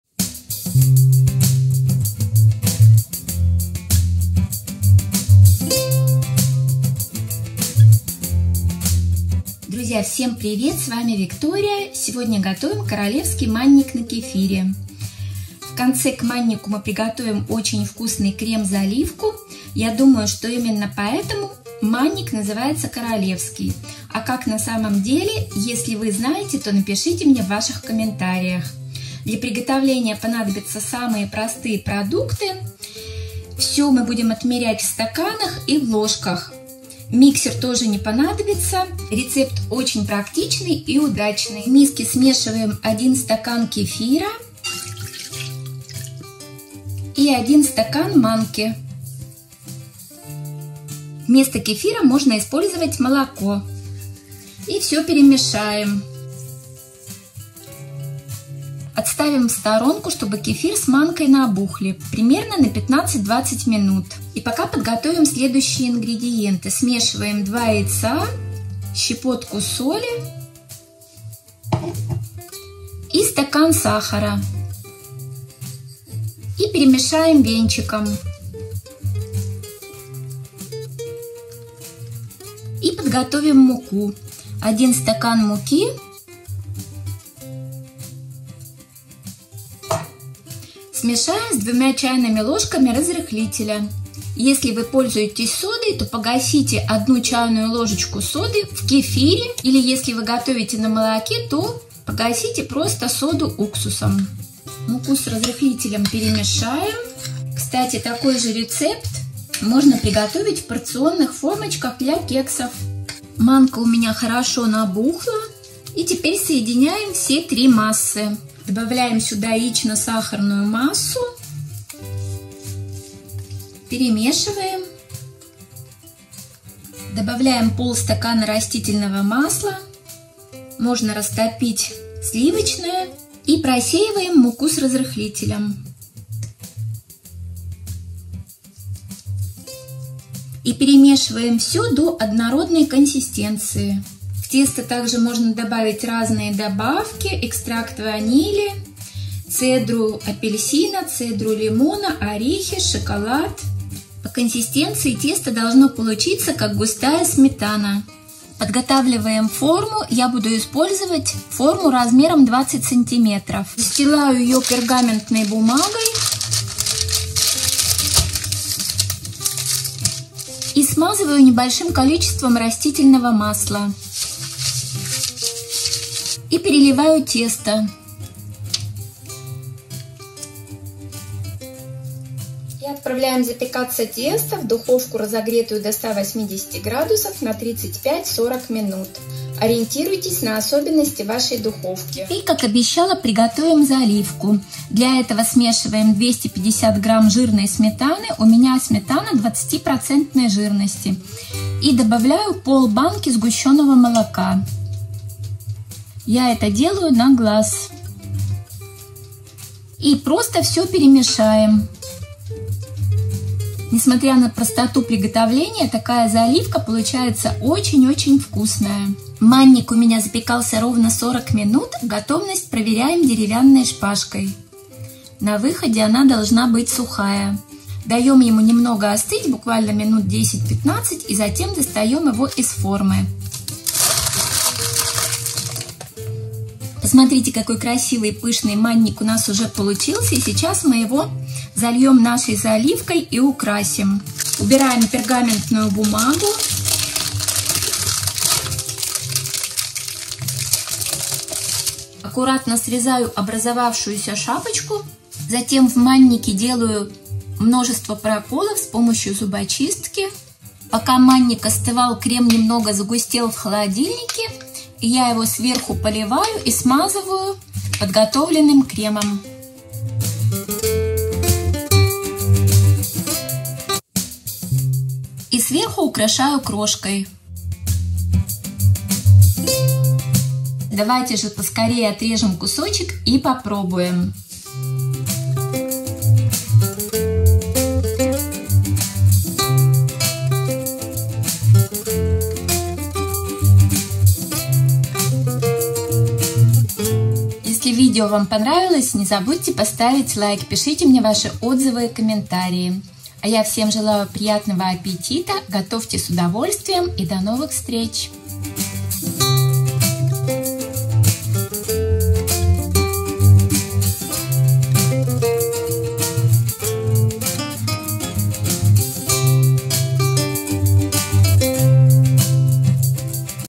Манник на кефире "Королевский" ! Быстро к чаю / Semolina Cake
Друзья, сегодня предлагаю приготовить Манник на кефире или Манник "Королевский" ! К маннику,  в конце, мы приготовим очень вкусный крем-заливку ! Прекрасный рецепт выпечки к чаю на скорую руку ! Кефир можно заменить на молоко.
Friends, today I propose to cook Semolina Cake or Mannik “Royal”! At the end, we will prepare a very tasty cream ! A wonderful recipe for quick baking for tea! Kefir can be replaced with milk.

МАННИК «КОРОЛЕВСКИЙ» ВКУСНАЯ ЗАЛИВКА !
Друзья, предлагаю вам рецепт очень вкусного манника с кремом/заливкой. В итоге получается домашний тортик на скорую руку, без возни и заморочек ! Прекрасно к чаю ! Очень Рекомендую !
Ингредиенты :
Стакан=250 мл
Манная крупа - 1 ст. ( 180г)
Сахар - 1 ст. (180г)
Кефир - 1 ст. ( 240 мл.) или молоко
Яйца - 2 шт.
Мука пшеничная - 1 ст. (180 г)
Растительное масло - 1/2 ст.
Разрыхлитель - 2 ч л неполные ( 8 г)или 1 ч л соды погасить в кефире
+Дополнительно вкусная заливка
Возможные добавки - ваниль, цедра лимона или апельсина, изюм, орехи, шоколад.